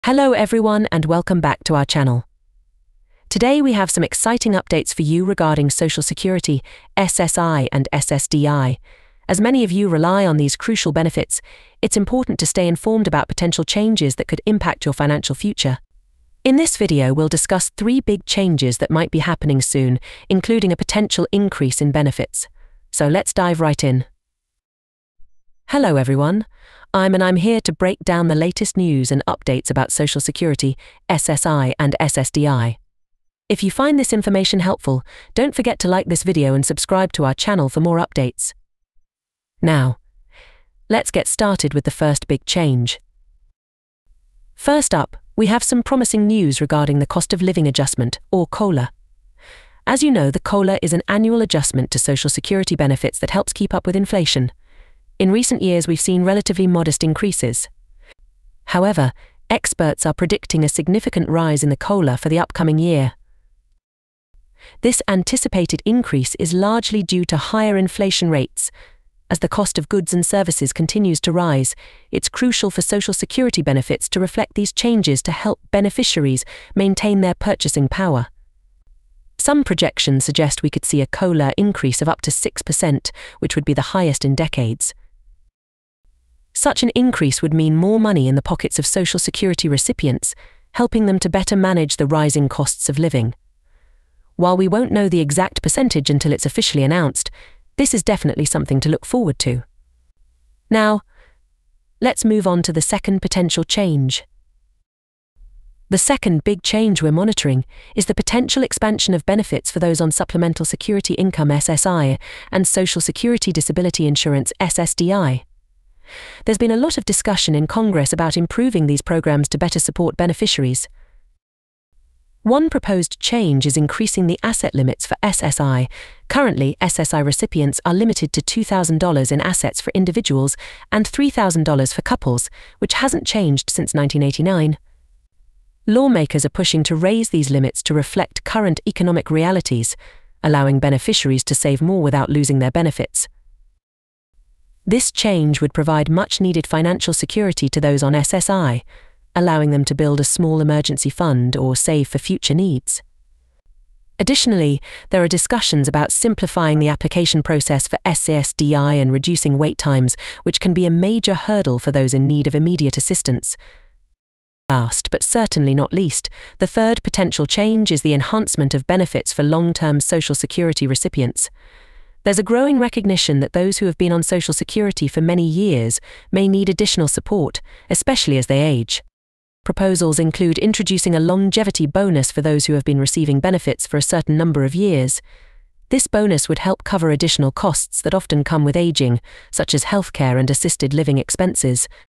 3 BIG CHANGES Possibly Happening Increase For Social Security, SSI, SSDI
Get ready for major updates! In this video, we discuss three significant changes that could potentially increase benefits for Social Security, SSI, and SSDI recipients. Learn about the proposed adjustments, who will be affected, and how these changes could improve financial stability for millions of Americans. Stay informed about the latest developments in government policies and understand what these potential increases mean for your benefits. Watch now to stay ahead and prepare for these important changes!